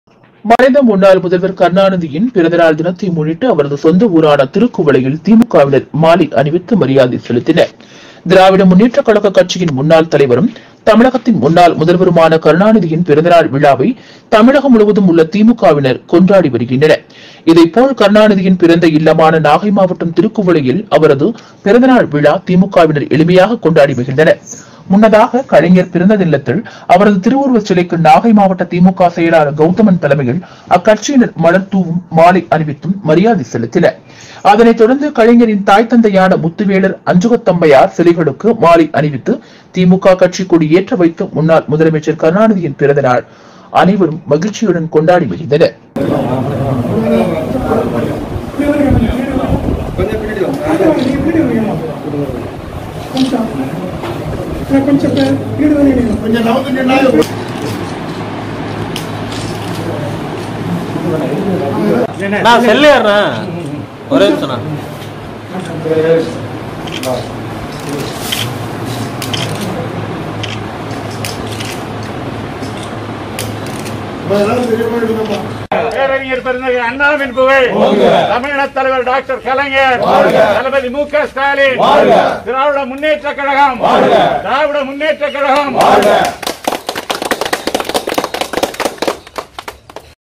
முதல்வர் கருணாநிதி 98 வது பிறந்தநாள் விழா திருக்குவளை திமுக கொண்டாட்டம்
மறைந்த முன்னாள் முதல்வர் கருணாநிதியின் 98, வது பிறந்தநாளை முன்னிட்டு அவரது சொந்த ஊரான திருக்குவளையில் திமுகவினர் மாலை அணிவித்து மரியாதை

திராவிட முன்னேற்றக் கழக கட்சியின் முன்னாள் தலைவரும். தமிழகத்தின் முன்னாள் முதல்வருமான, கருணாநிதியின் 98,வது பிறந்தநாள் விழாவை தமிழகம் முழுவதும் உள்ள திமுகவினர் இன்று கொண்டாடி வருகின்றனர். இதைப்போல் கருணாநிதி பிறந்த இல்லமான நாகை மாவட்டம் திருக்குவளையில் அவரது 98,வது பிறந்த நாள் விழாவினை திமுகவினர் எளிமையாக கொண்டாடி மகிழ்ந்தனர். முன்னதாக கலைஞர் பிறந்த இல்லத்தில் அவரது திருவுருவ சிலைக்கு நாகை மாவட்ட திமுக செயலாளர் கெளதமன் தலைமையில் அக்கட்சியினர் மலர் தூவியும் மாலை அணிவித்தும் மரியாதை செலுத்தினர். அதனைத் தொடர்ந்து கலைஞரின் தாய் தந்தையான முத்துவேலர் அஞ்சுகத்தம்மையார் சிலைகளுக்கு மாலை அணிவித்து, திமுக கட்சி கொடி ஏற்றிவைத்து முன்னாள் முதல்வர் கருணாநிதியின் பிறந்தநாளை கொண்டாடி மகிழ்ந்தனர்.